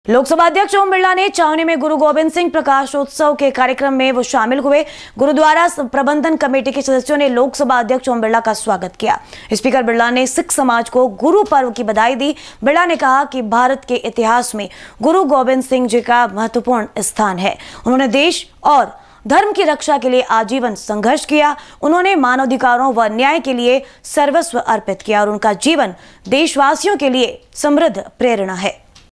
STN news 17-Jan -24 @ कोटा -कोटा संसदीय क्षेत्र कोटा बूंदी प्रवास पर स्पीकर ओम बिरला
कोटा  संसदीय क्षेत्र कोटा बूंदी प्रवास पर स्पीकर ओम बिरला